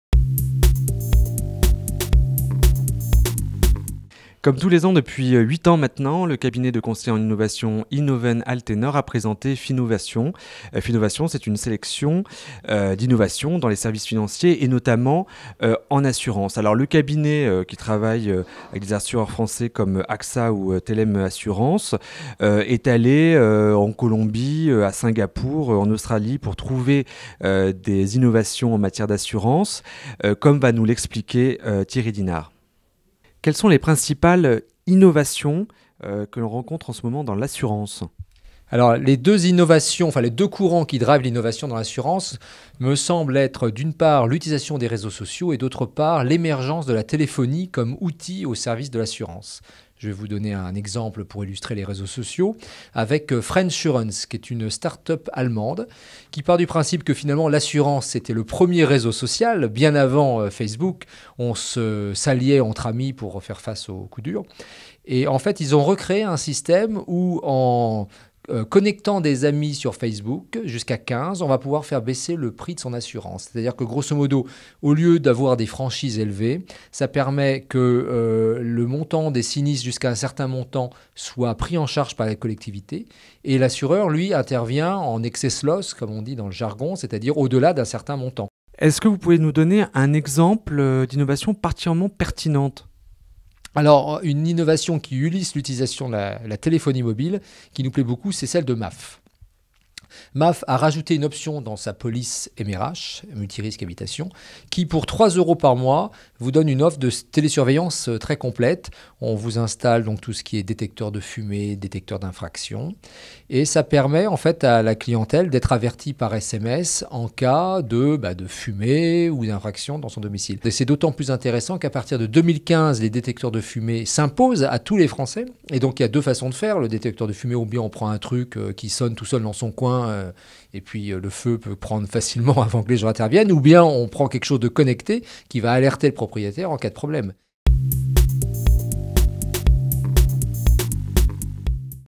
Innovation : les assureurs jouent le collectif
Cette tendance au « collaboratif » devrait se renforcer, selon le cabinet InnovenAltenor.